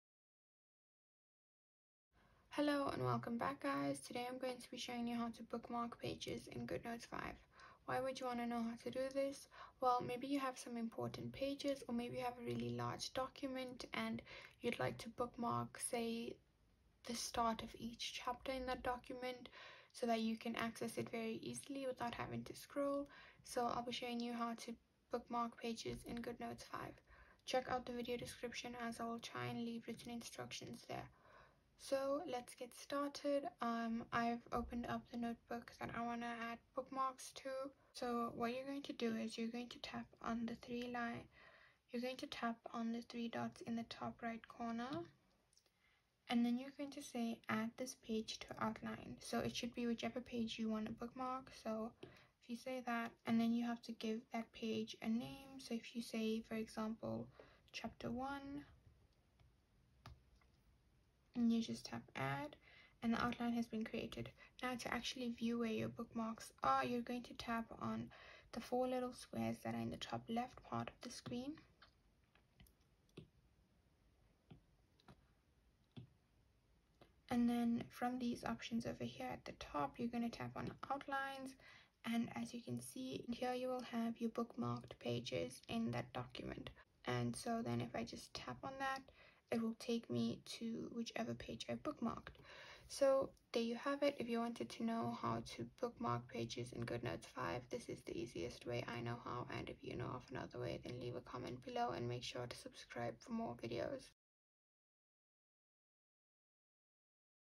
✅ How To Bookmark Pages In GoodNotes 5 🔴
About this product:

oodNotes 5, the latest version of the app, offers an updated user interface, improved search functionality, and additional features.